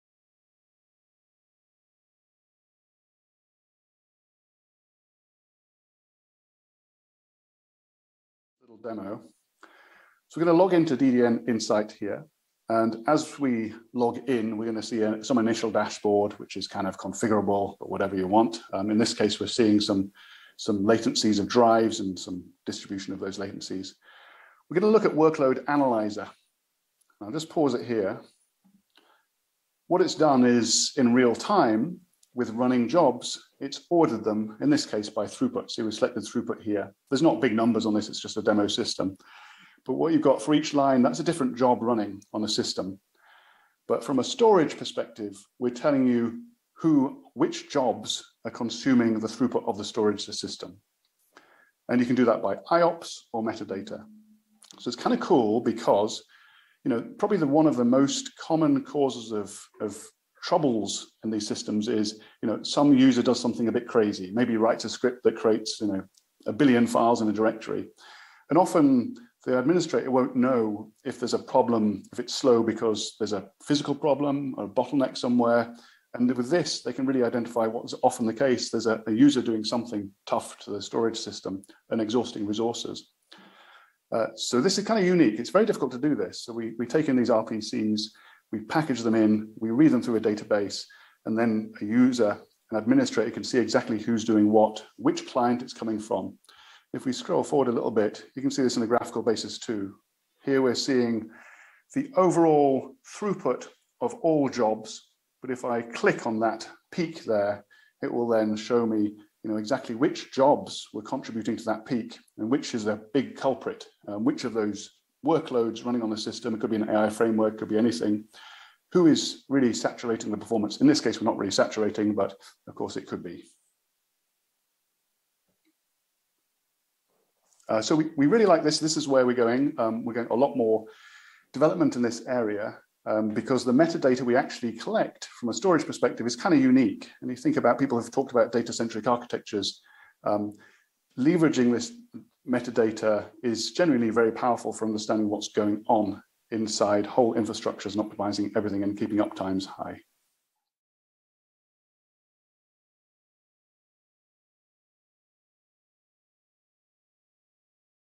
DDN Insight and Workload Analyzer Demo
Cut through the complexity to find actionable insights, guided by powerful graphical visualizations. DDN’s Insight supplies complete end to end visibility of AI workloads. Run your service more efficiently, more of the time, with the first complete view of both your applications and infrastructure with DDN Insight’s Workload analyzer. Presented by James Coomer, Sr. VP of Products, DDN.

Recorded in Santa Clara, CA on May 18, 2022 as part of AI Field Day 3. for more information.